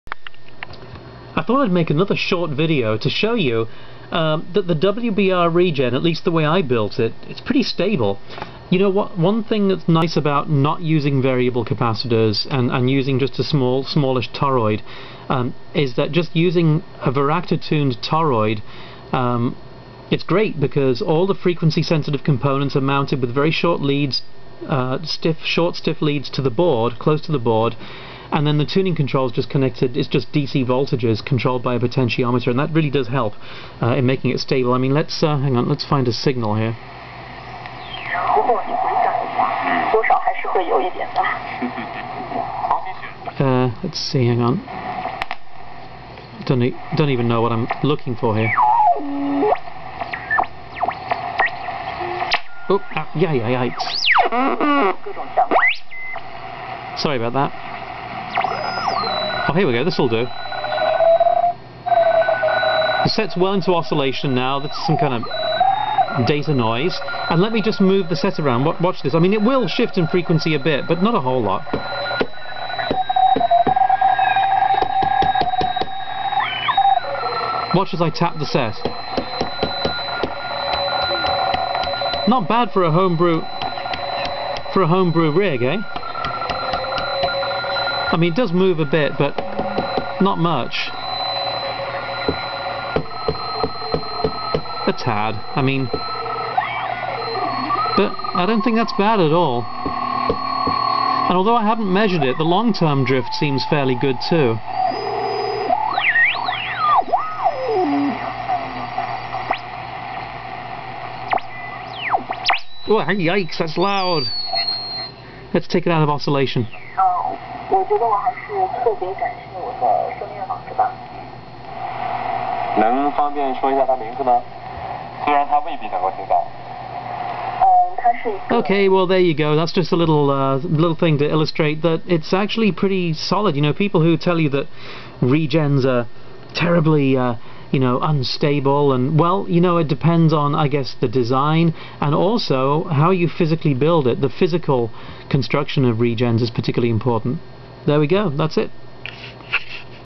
The Stability Of A Homebuilt Regen - The WBR
Rigid physical construction is important for a homebrew regen receiver. This short video shows how stable this build of the WBR Regen Receiver is. I could have made it more impervious to sharp knocks by using nylon screws and washers to firmly hold the toroid above the board, but depending on the exact material used, this could have introduced some drift into the circuit. I chose instead to wind the toroid with relatively stiff 22 gauge wire and make it self-supporting.